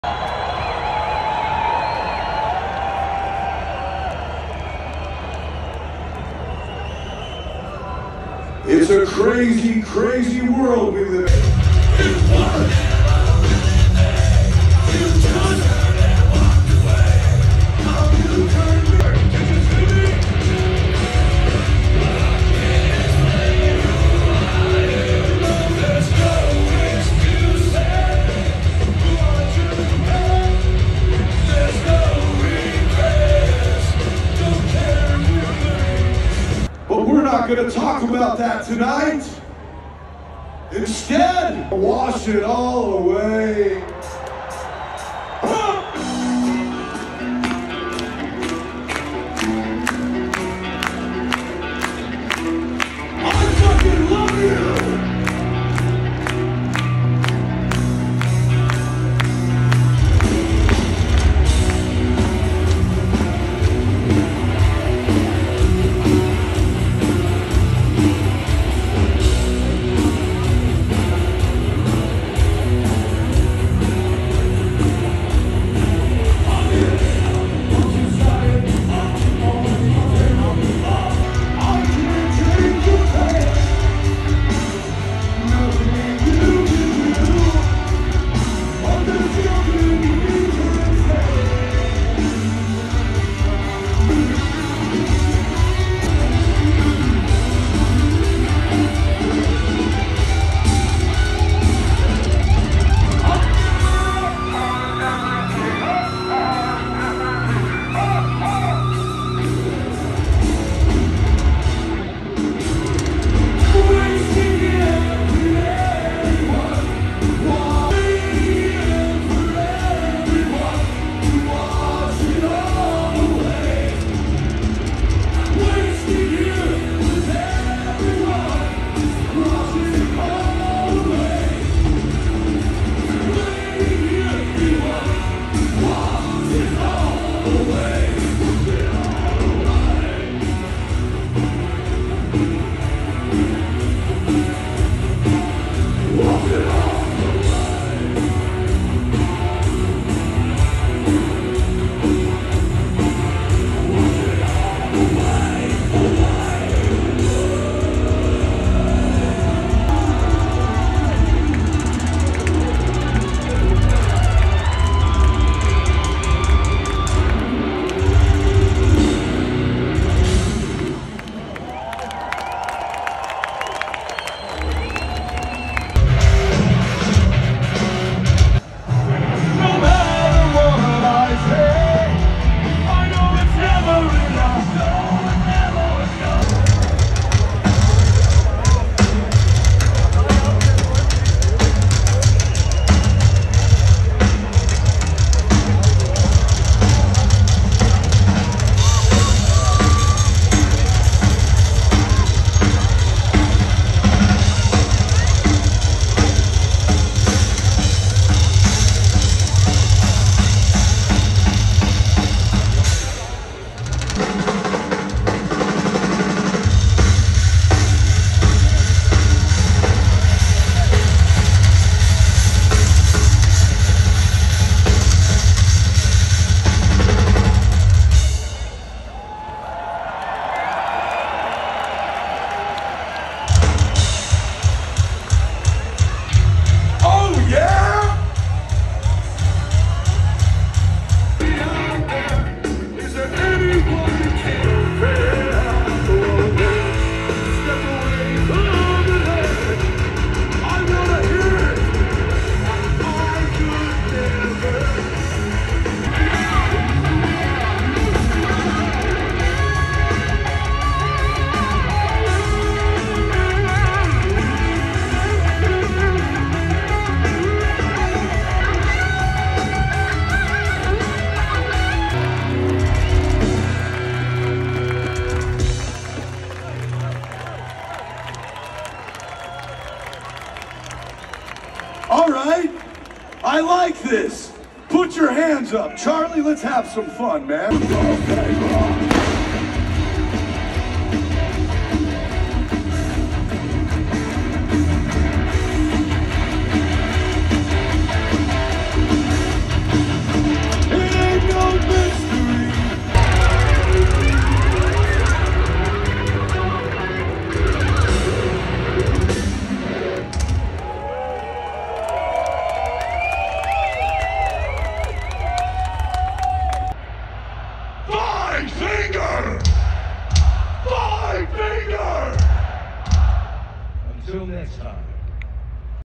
Five fingers death punch - OUT IN THE GREEN
Heute seht ihr das erste outinthegreen nach 25 Jahren.
Eigentlich, war metallica gebucht gewesen die hatten auch ihren soundcheck gemacht aber durch einen Fall im Team von corona fiel der Auftritt aus.